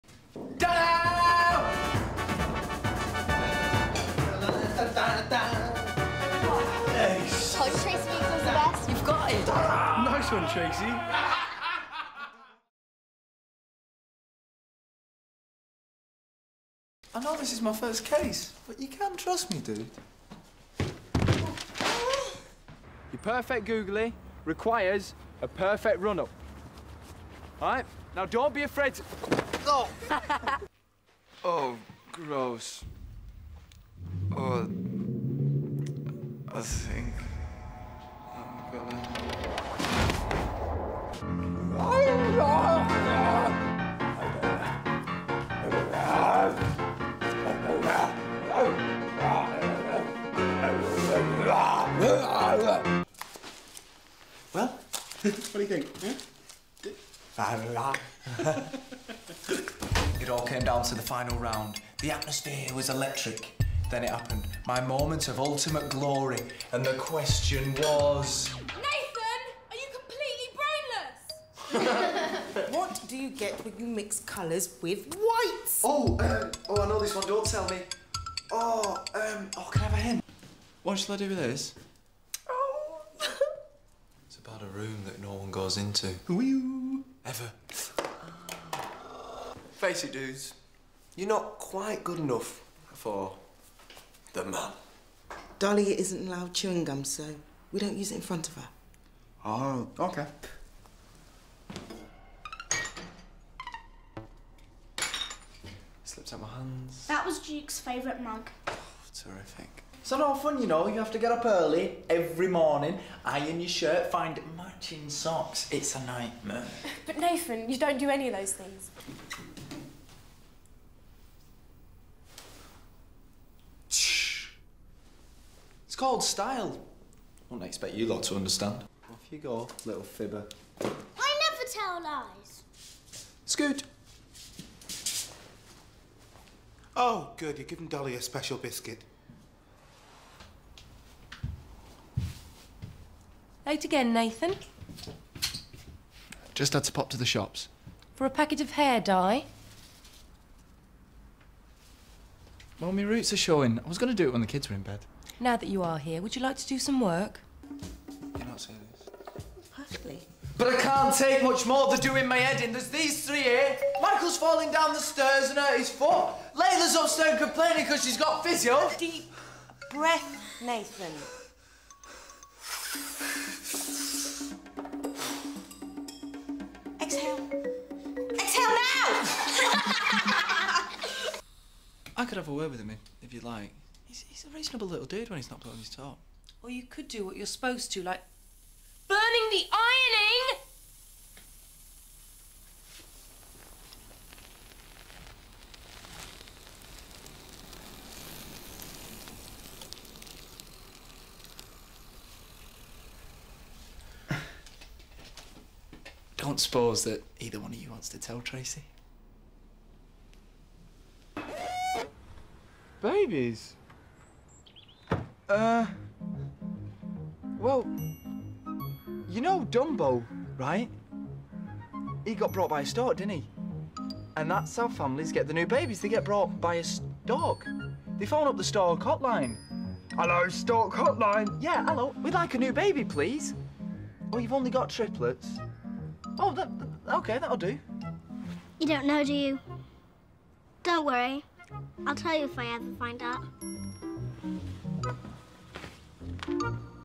nathan being a disaster for 4 minutes straight
Nathan being a terrible careworker for 4 minutes.